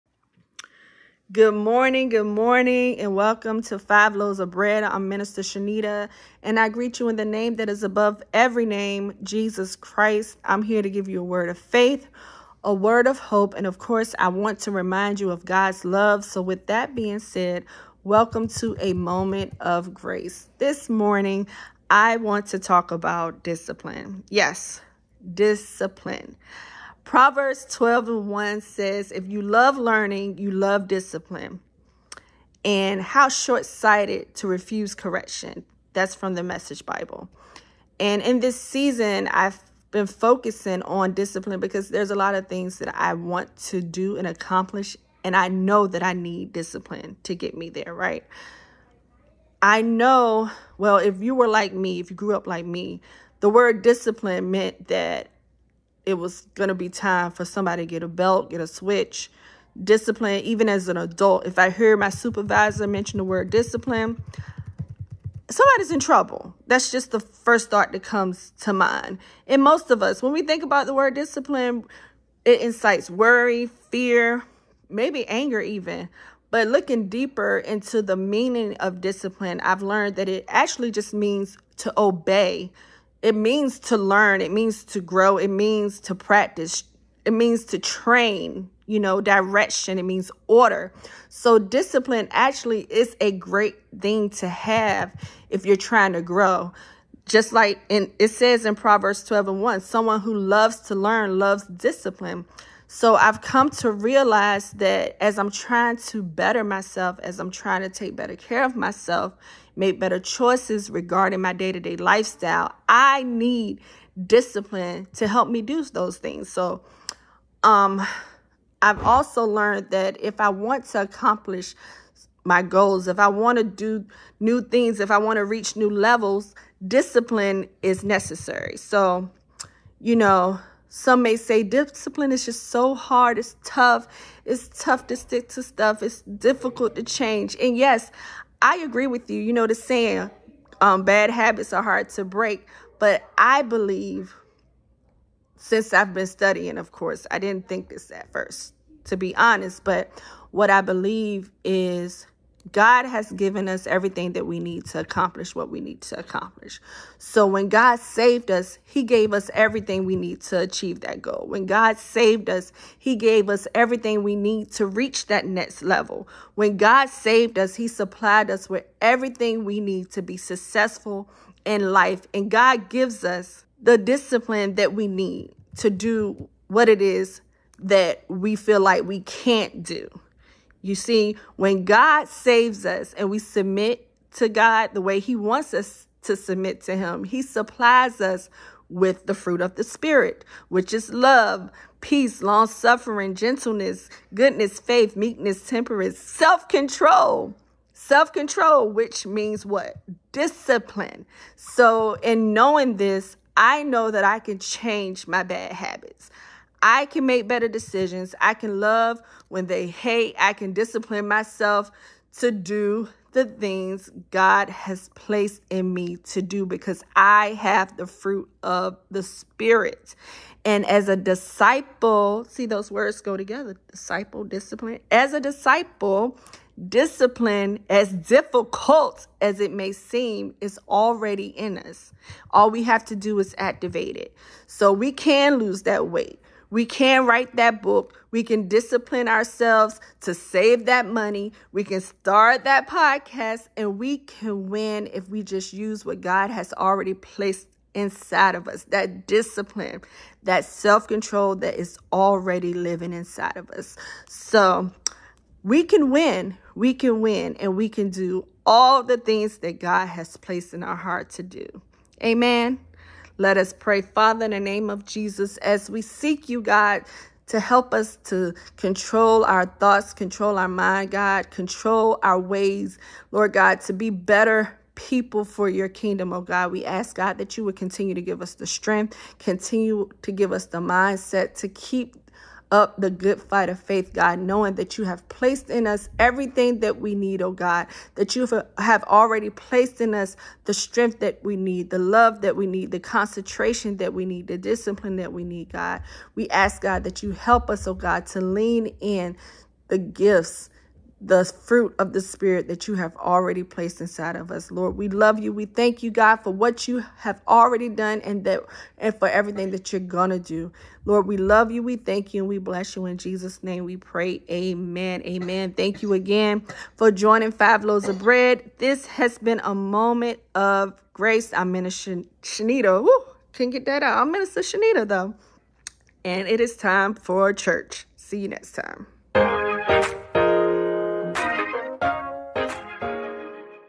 A Moment of Grace Ep 33
This is a weekly message to encourage you to not fear discipline but to rely on God to give you the discipline you need to get things done. Be a disciplined disciple of God.